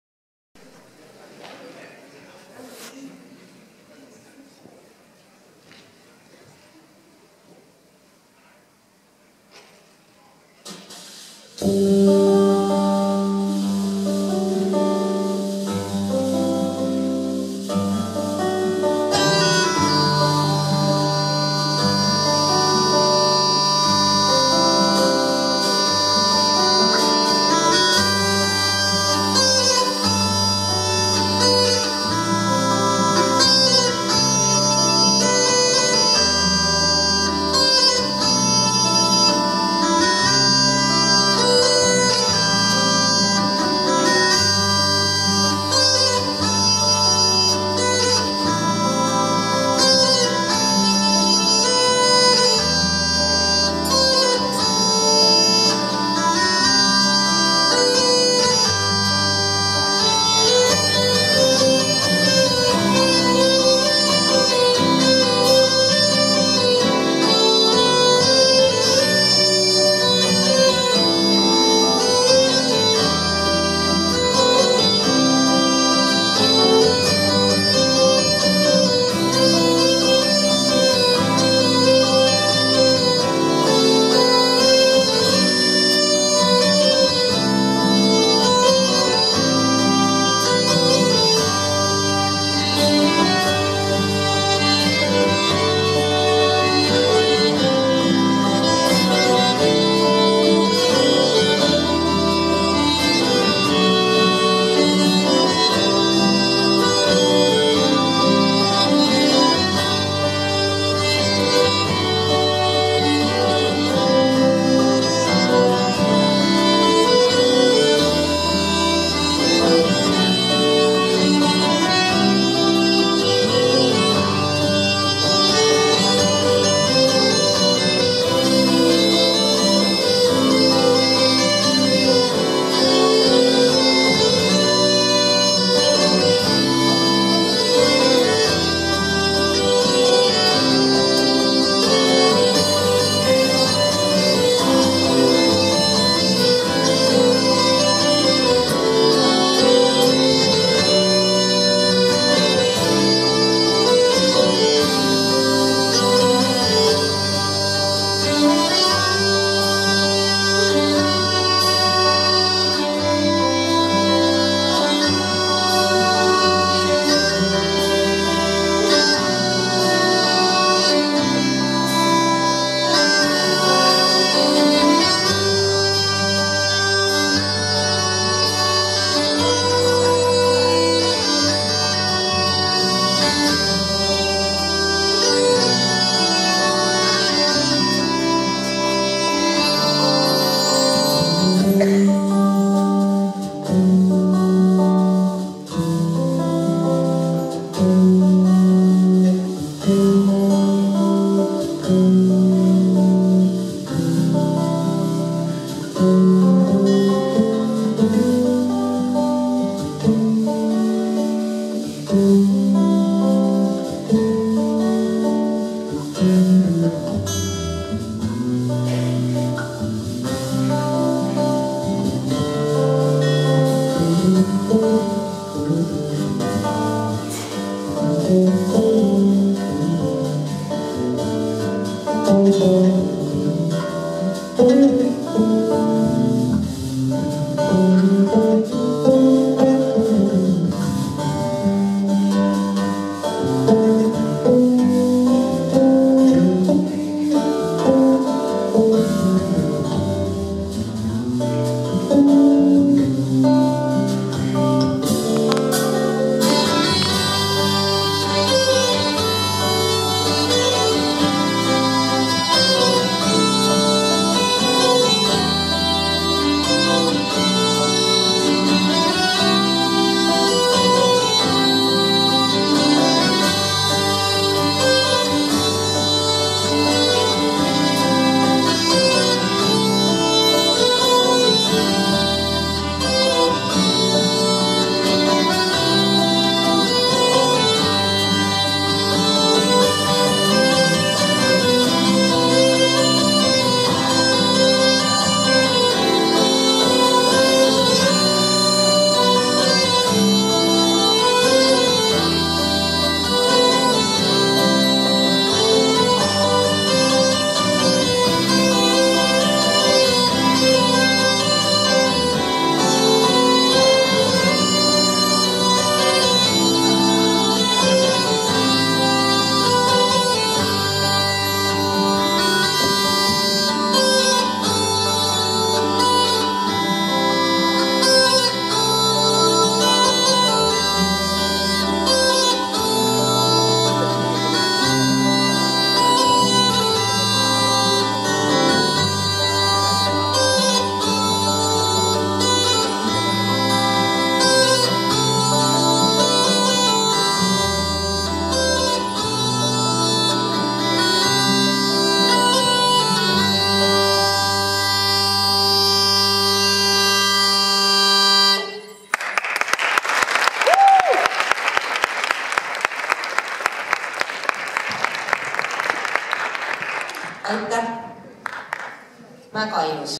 I ülevaatus Kagu Kabujalakõsõ I ja II Puudutuse aeg
I ettetantsimine Põlvamaal.
Tantsib: Kagu Kabujalakõsõ, juhendajad: Maido Saar, Aveli Asber, Andre Laine